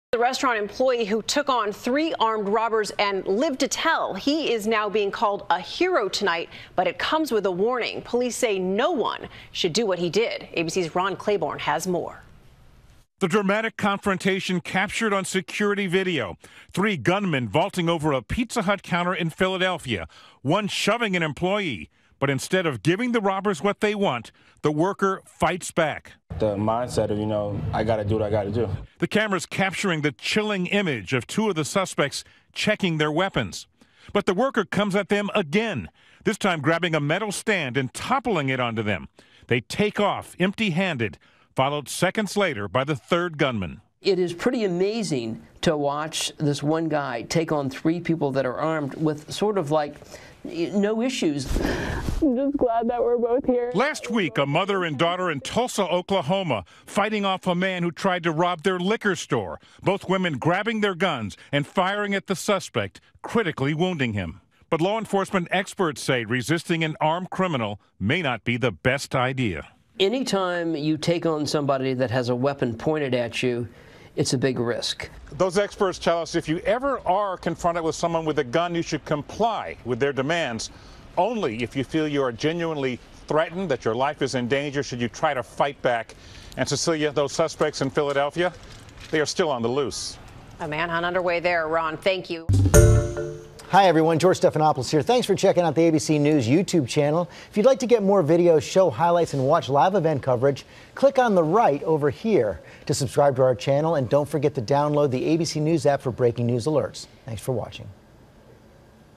Restaurant Employee Takes On Three Gunmen In Attempted Robbery
March 03, 2018 - Law enforcement experts suggest complying with a gunman's demands unless the situation is life-threatening; in which case, they suggest people fight back.